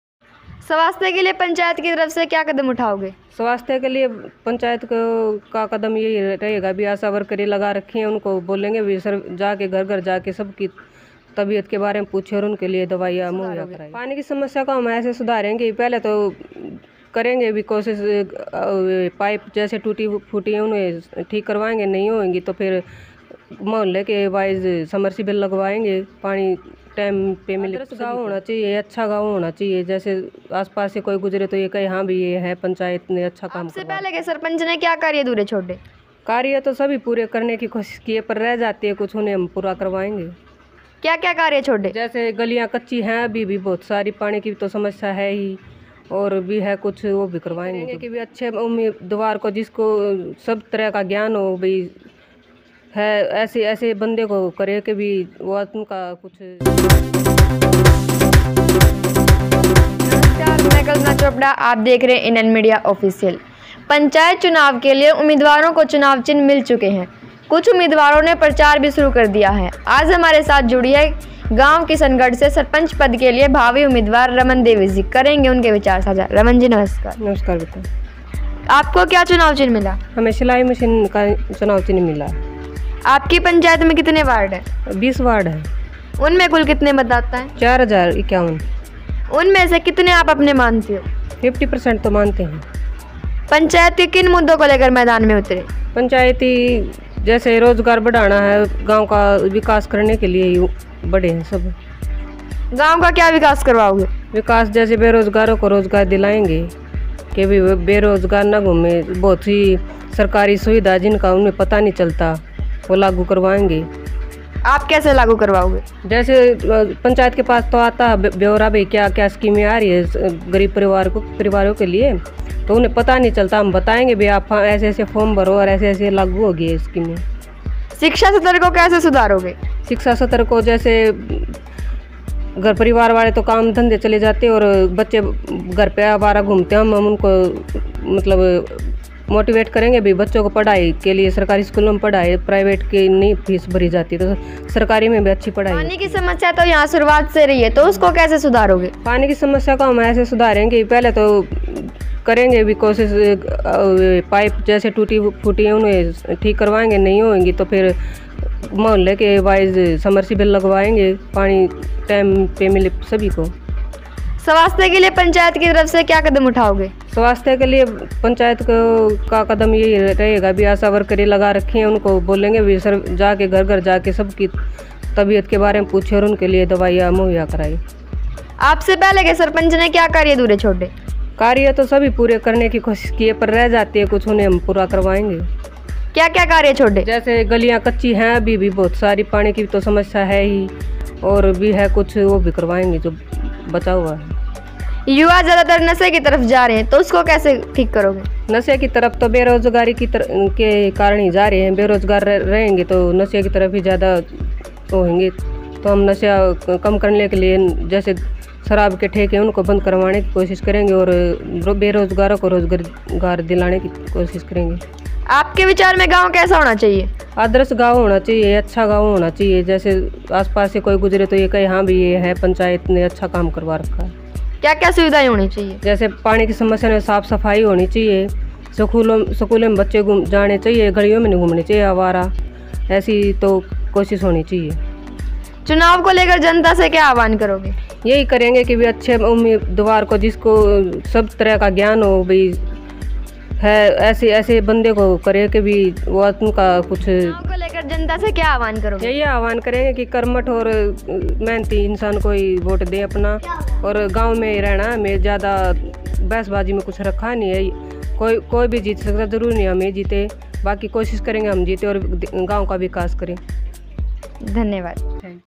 Meham~अब समस्याएं होगी छुमंतर ! मुखिया पद की दमदार उम्मीदवार रमन देवी ने ठोकी ताल
Meham~अब समस्याओं से मिलेगी निजात ! मुखिया पद की दमदार उम्मीदवार रमन देवी ने ठोकी ताल